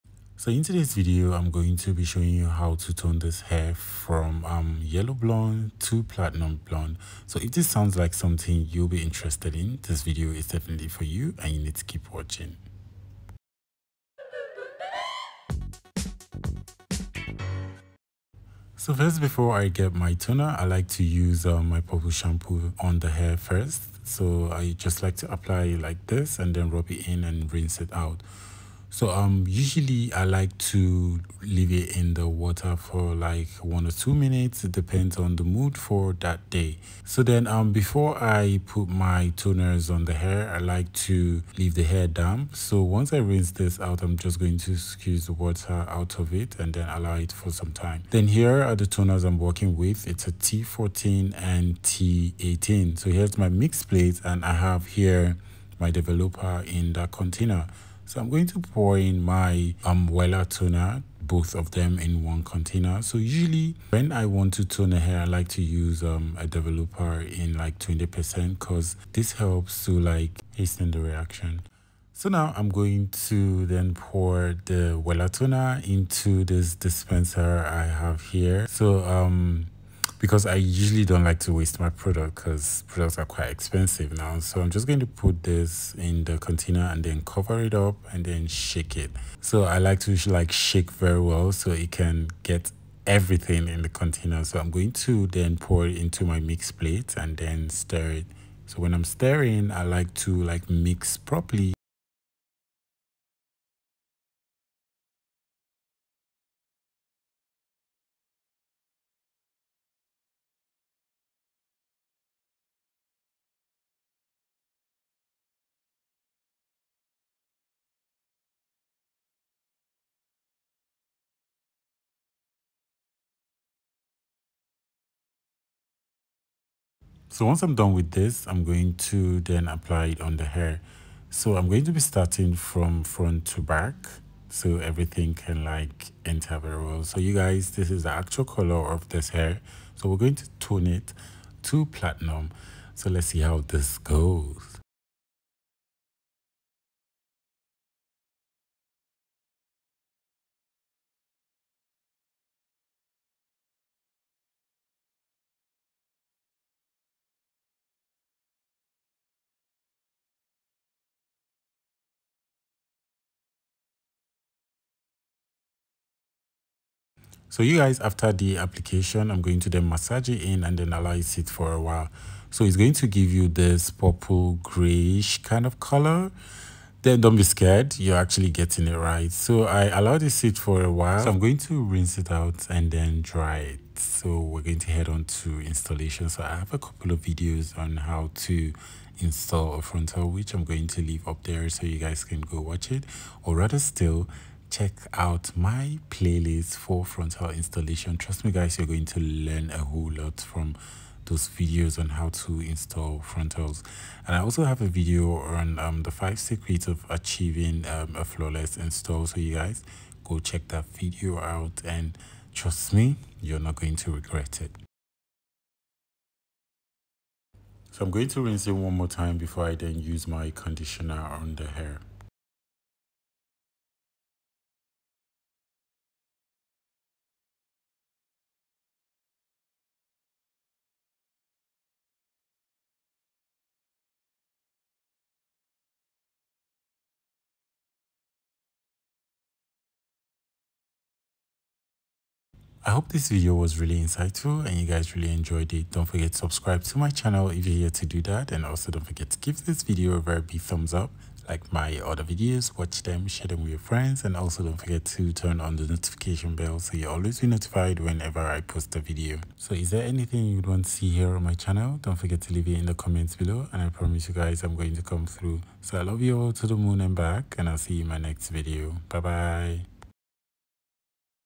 HOW TO TONE HAIR | brassy to ash blonde wella toner
In today’s video I’ll be showing you how to correct brassiness using wells toner in T14 and T18

Want to take a private course on hair styling and how to install a frontal?